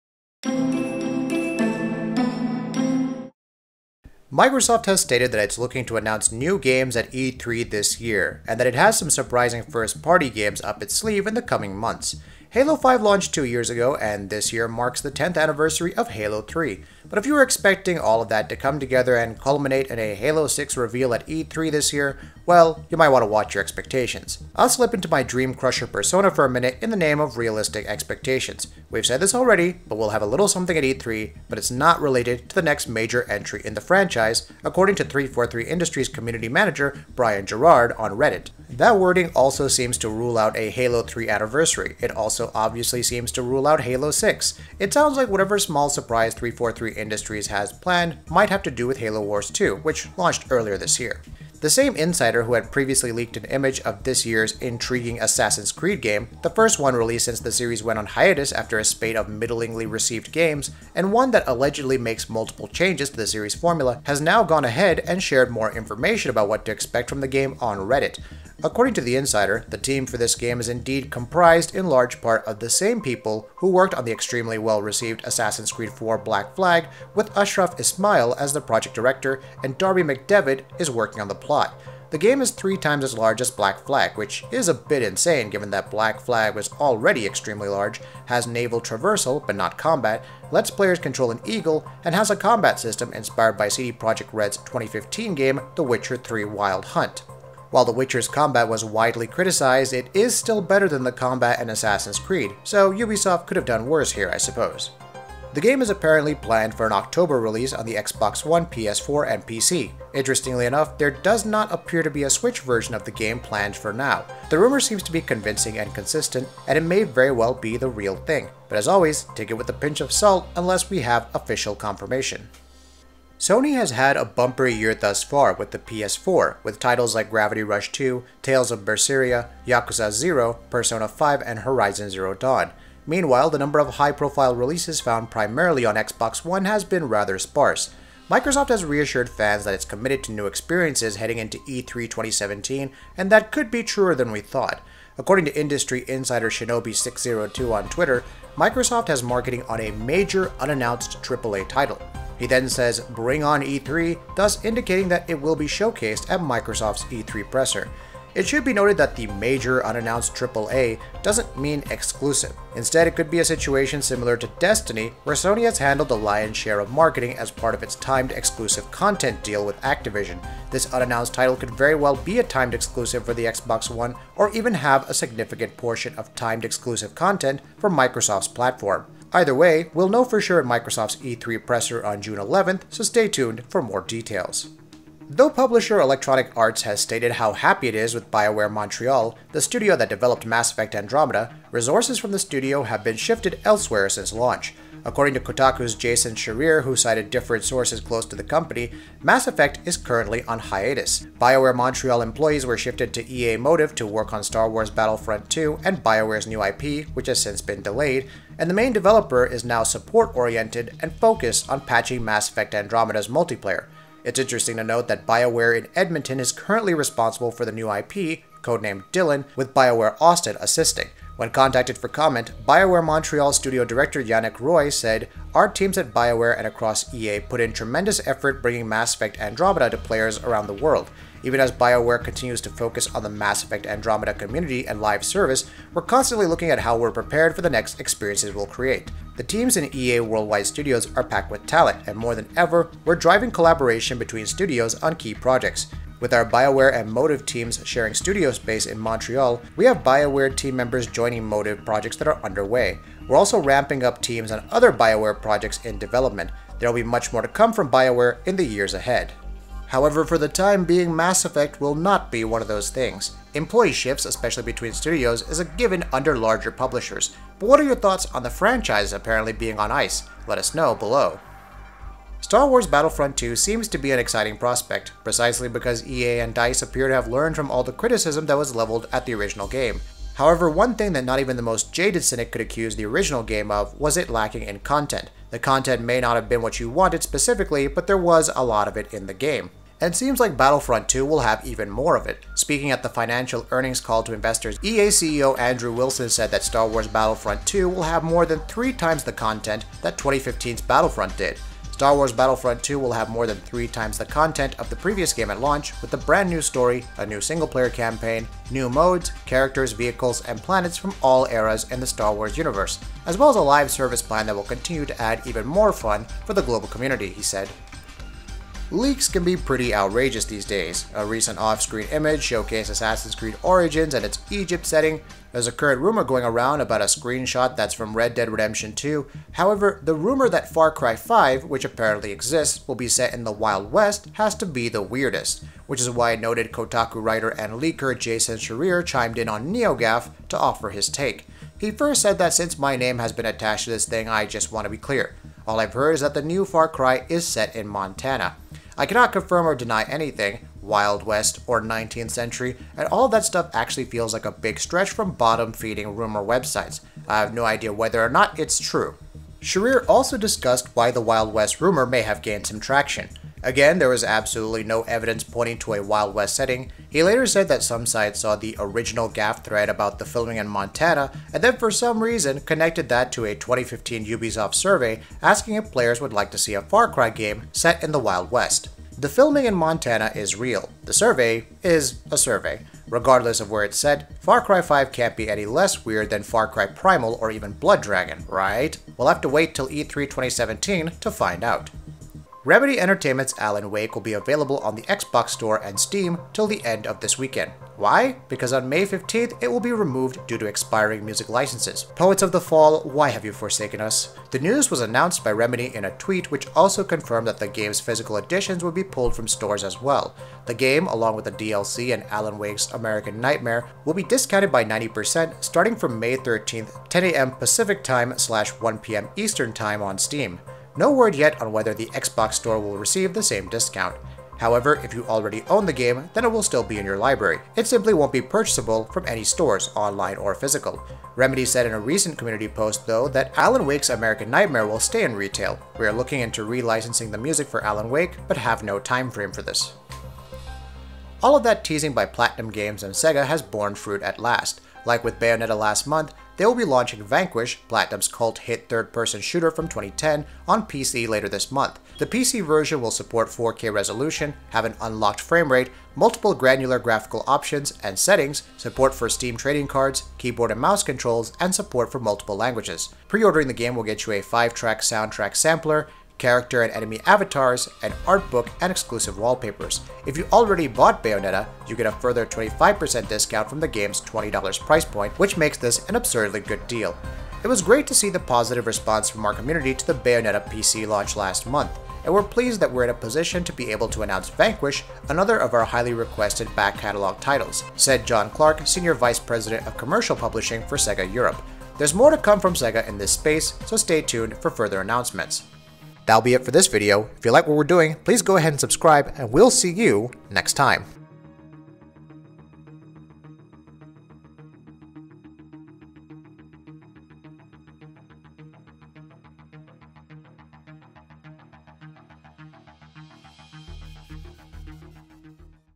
New Assassin's Creed Is 3 Times Bigger Than AC4, No Halo 6 At E3, No New Mass Effect, Far Cry 5 Info
Today we cover the following news in this video:

Halo 6 Won't Be Announced At E3 2017
Assassin's Creed Origins Will Be Three Times Bigger Than Black Flag
Microsoft Possibly Has Exclusive Marketing Rights For A Major Unannounced AAA Game
There Is No One Working On A New Mass Effect Game
Star Wars Battlefront 2 Has Three Times The Content Found In 2015's Game
Far Cry 5 Is Possibly Set In Montana
Alan Wake Is Being Pulled From Sale Due To Music Licensing Issues
Vanquish Launches On PC On May 25